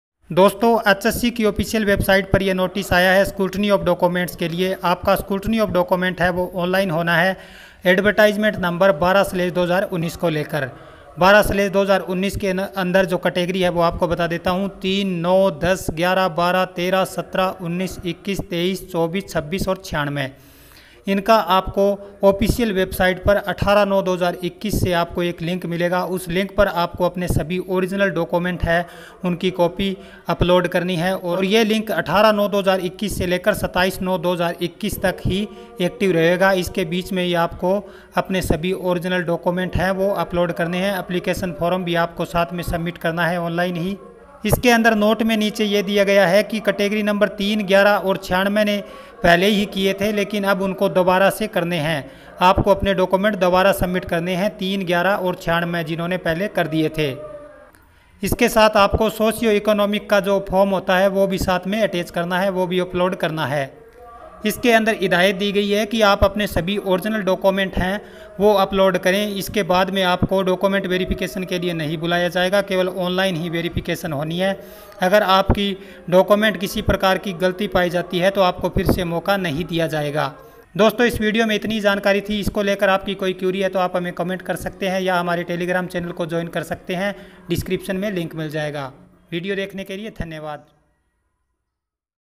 HSSC 12/2019 Latest Update 2021 | Hssc 12/2019 Latest News
dosto hssc 12/2019 latest update ke anusar ab aap ko various category ka dv form online apply karna hoga. jin ne hssc 12/2019 ka pahel document upload kiye the unhe fir se karne hai.